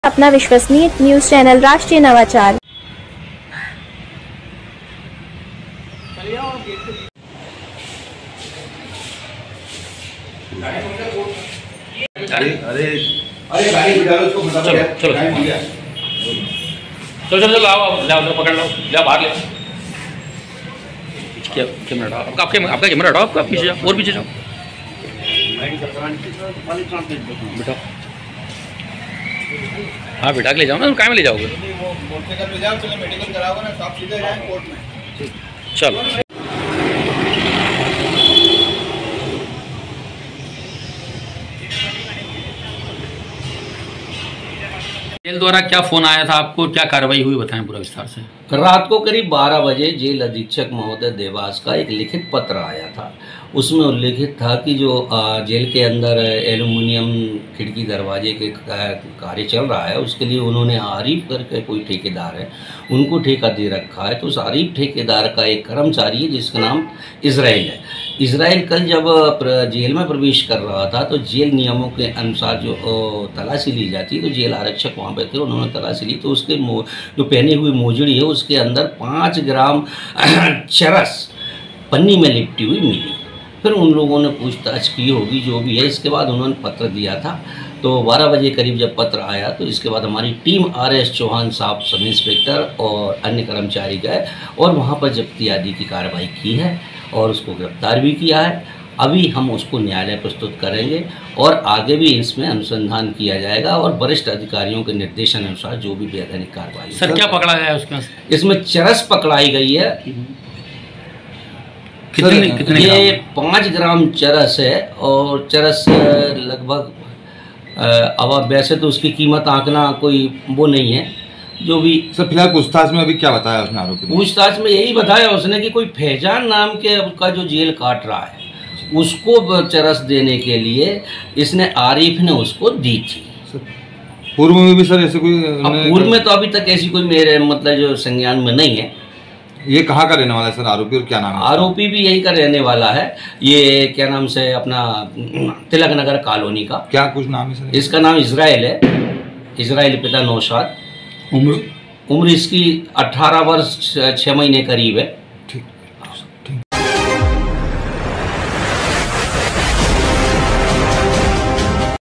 जेल में 5 ग्राम चरस ले जा रहा था इसराइल नामक युवक बंद कैदी फैजान के लिए आरिफ ठेकेदार की मदद से।
जेल में 5 ग्राम चरस ले जा रहा था इसराइल नामक युवक जेल में बंद कैदी फैजान के लिए आरिफ ठेकेदार की मदद से।